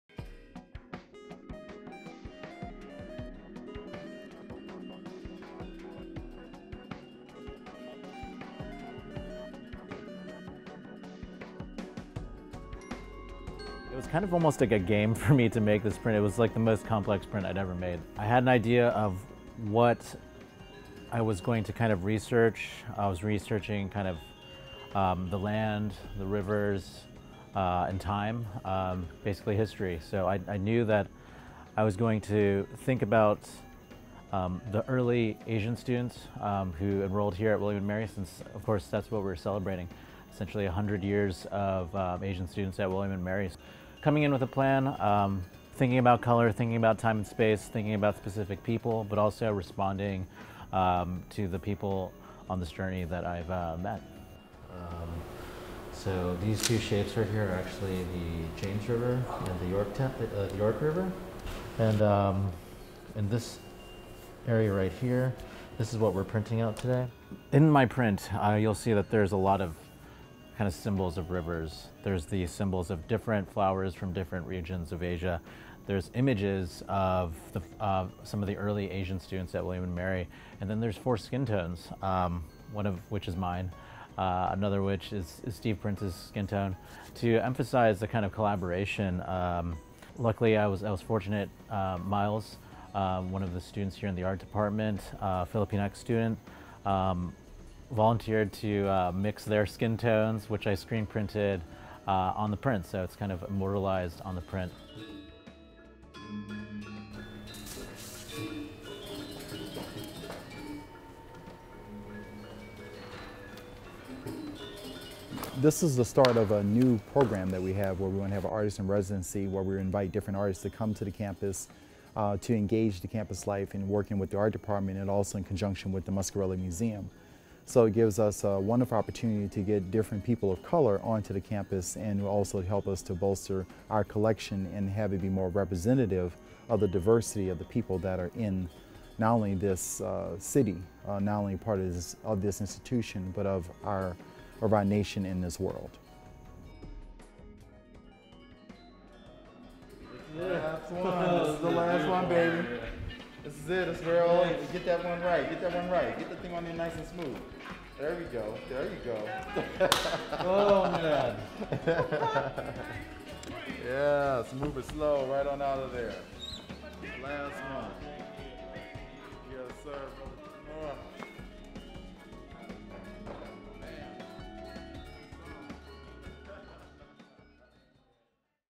'Confluence': Creating the Asian Centennial print
Artist Roberto Jamora discusses the making of 'Confluence,' which commemorates the earliest Asian American students in attendance at William & Mary.